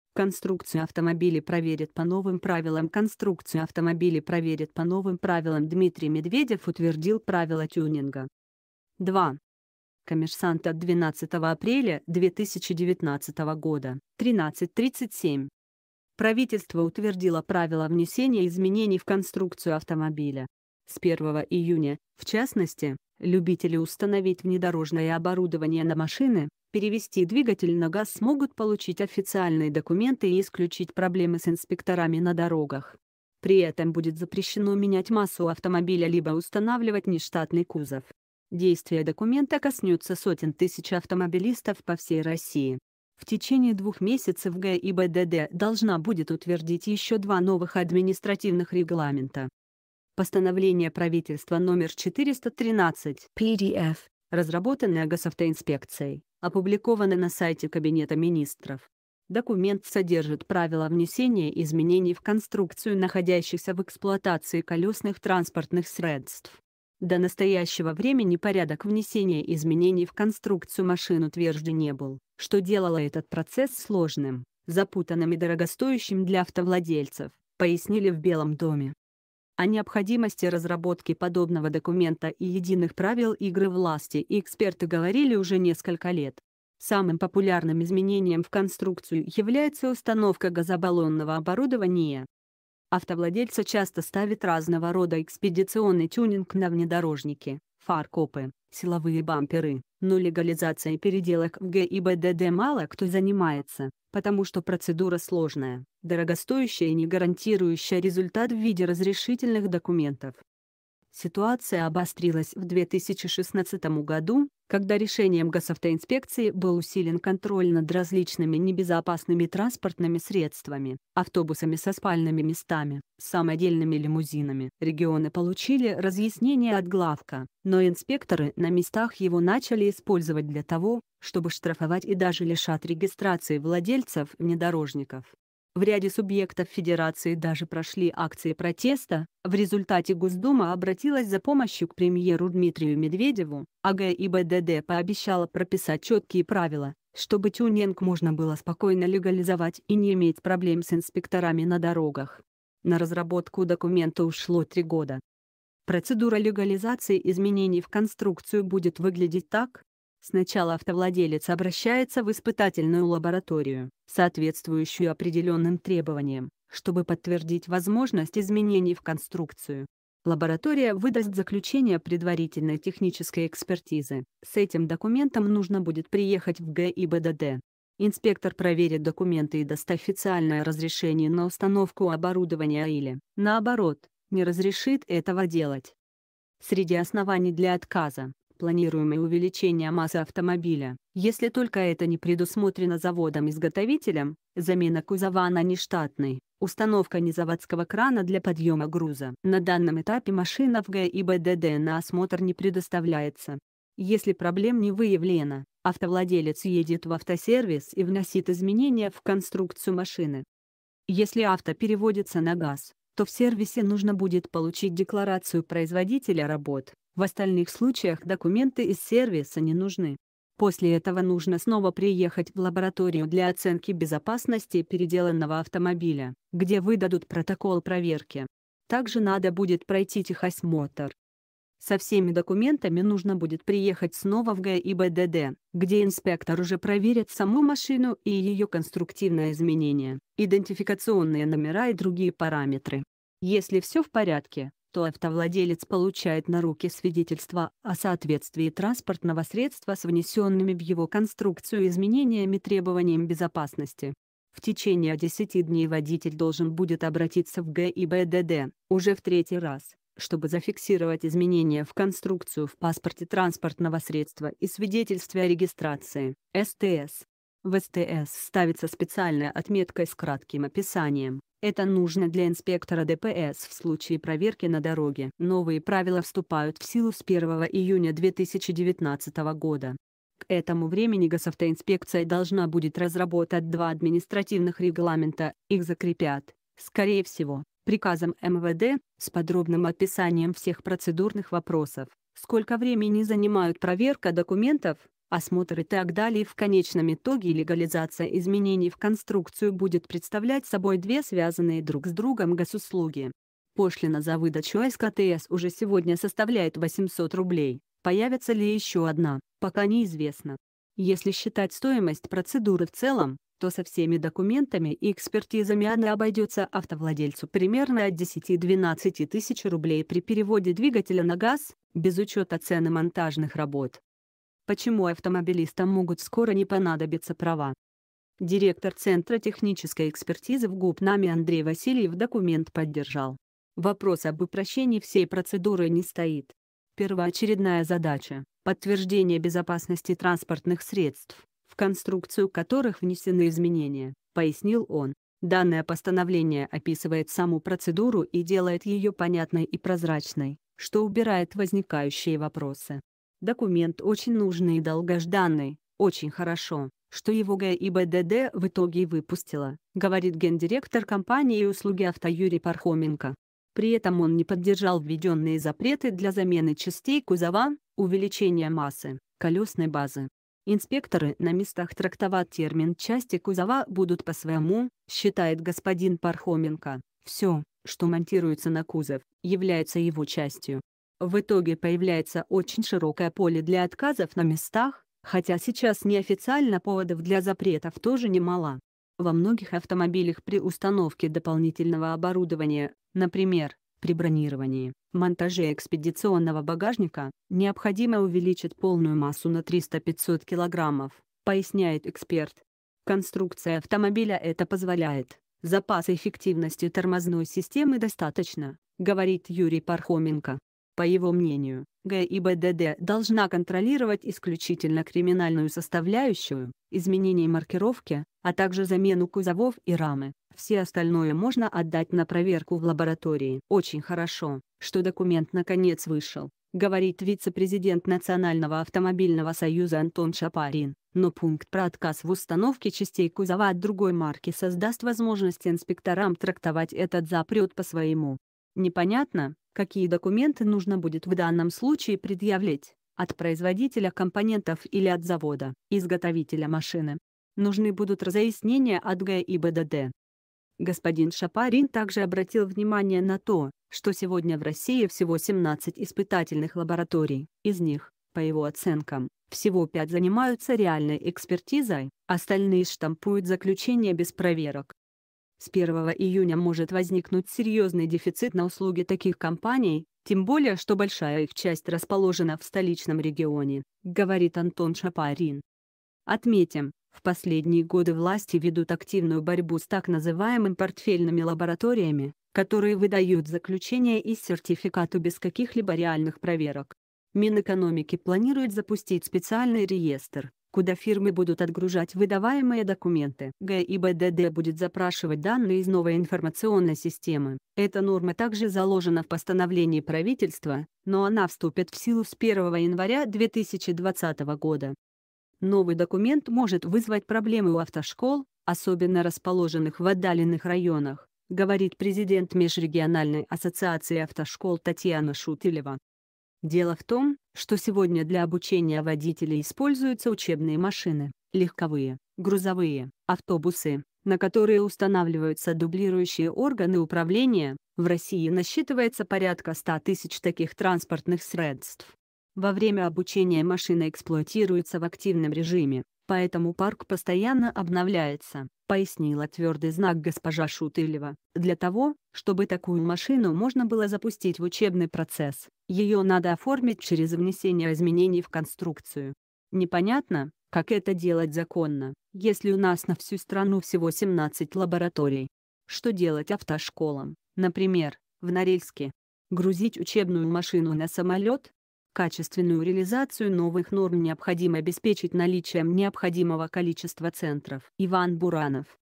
Конструкцию автомобилей проверят по новым правилам
Конструкцию автомобилей проверят по новым правилам Конструкцию автомобилей проверят по новым правилам Дмитрий Медведев утвердил правила тюнинга 2 Коммерсантъ от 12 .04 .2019, 13 37 Правительство утвердило правила внесений изменений в конструкцию автомобиля . С 1 июня, в частности, любители установить внедорожное оборудование на машины, перевести двигатель на газ смогут получить официальные документы и исключить проблемы с инспекторами на дорогах . При этом будет запрещено менять массу